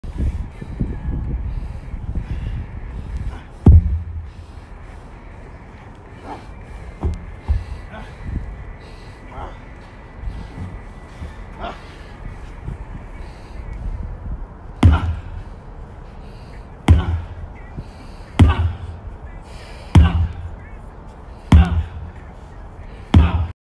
Boxing motivation
Tractor tire flips + sledge hammer slams